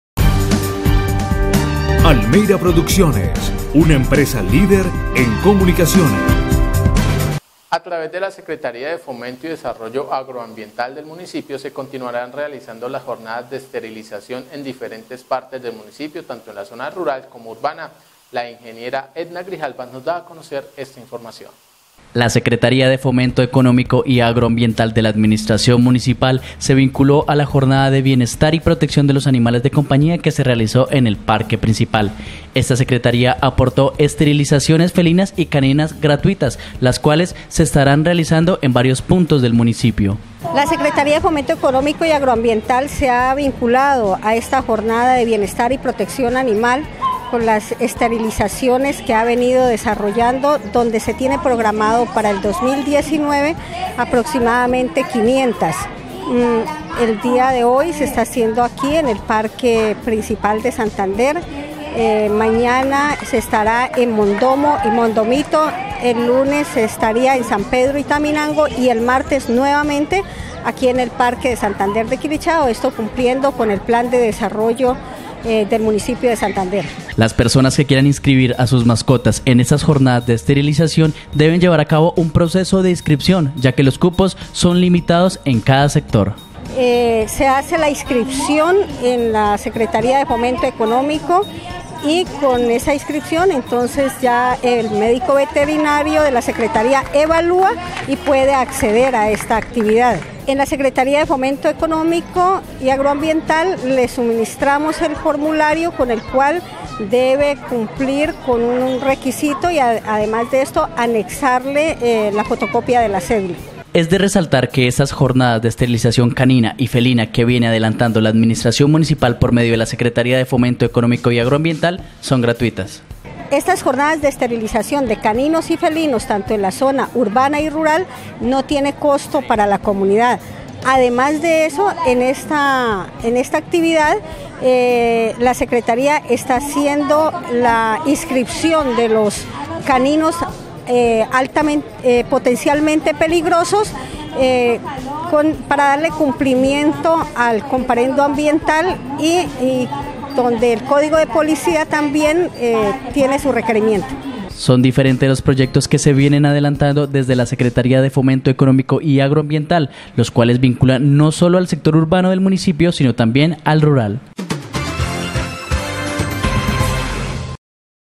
ALCALDIA CONTINUA JORNADA DE ESTERILIZACION
CONTINÚAN LAS JORNADAS DE ESTERILIZACIÓN CANINA Y FELINA

La Secretaria de Fomento Económico y Agroambiental, la Ingeniera Edna Grijalba, nos da a conocer las jornadas de esterilización en felinos y caninos que viene adelantando la Administración Municipal a través del Plan de Desarrollo de Santander de Quilichao.